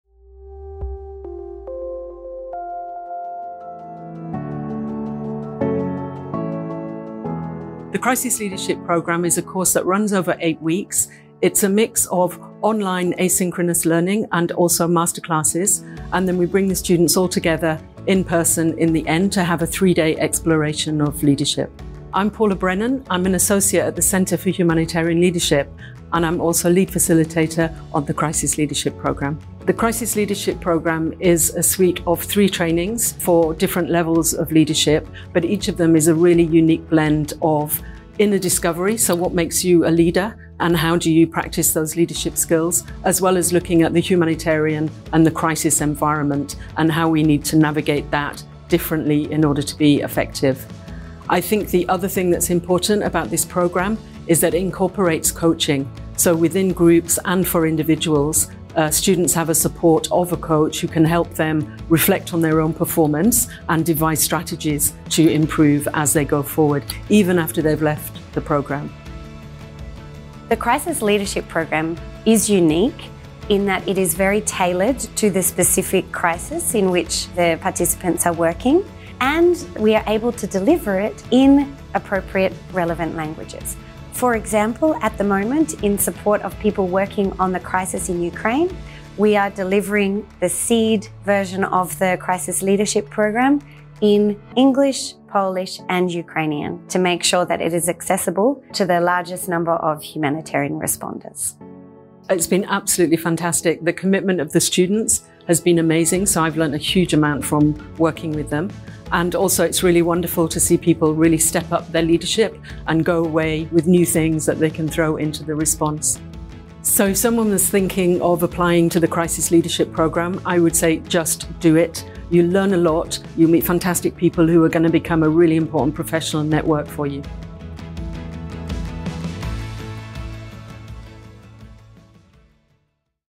Crisis Leadership Program | About the CLP
In this video, Rachel Routley and Paula Brennan from the Centre for Humanitarian Leadership explain all about the Crisis Leadership Program (CLP).

The Crisis Leadership Program (CLP) offers hands-on short courses with flexible delivery models to meet the needs of first responders working in crisis response and recovery.

The CLP is managed by the Centre for Humanitarian Leadership and supported by the Center for Disaster Philanthropy, the Humanitarian Leadership Academy and Save the Children. The project is funded with UK aid from the UK government.